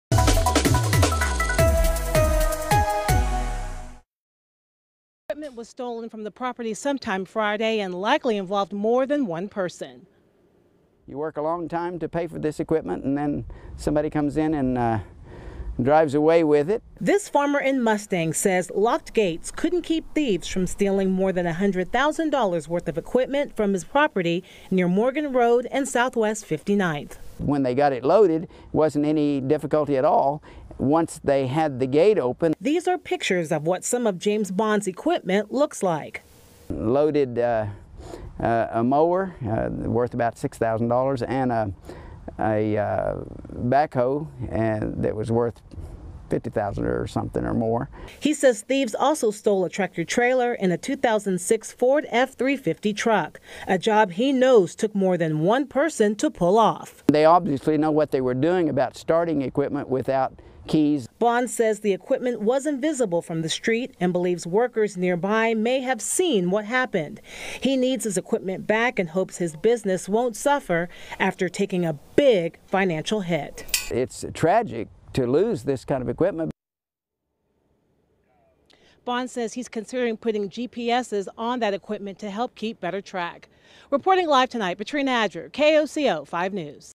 Farmer says locked gates couldn’t stop thieves from taking $100,000 in equipment
Someone or a group of people stole about $100,000 worth of equipment from a Mustang man’s farm late last week.